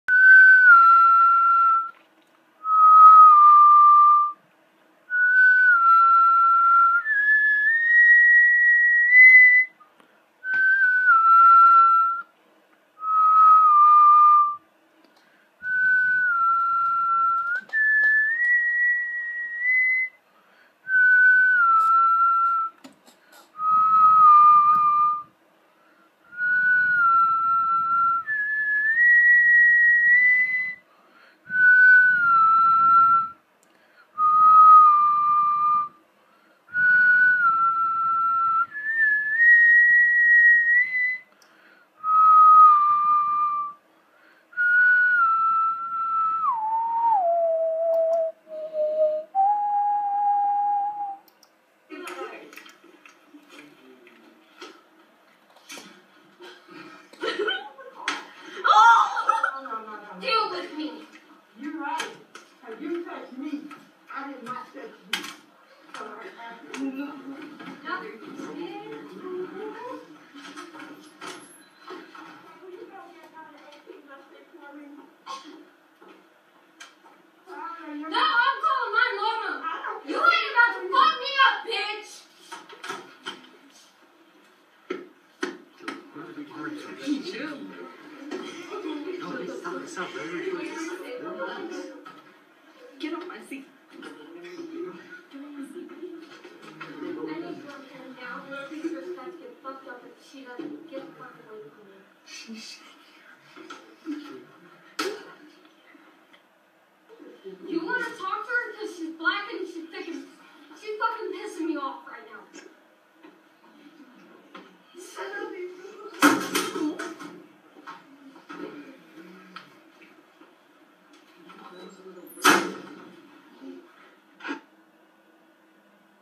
White Privilege All Over The Place
So in the State of Texas, Fort Worth, Texas, a altercation between a white student, and a black teacher ensued, when the student slaps the teacher on the arm, calling her mom on the teacher, and slams the phone on the teacher's desk. Thinking because she black that the student can't have the teacher fired. so she storms out of the classroom. This took place in Castleberry High School.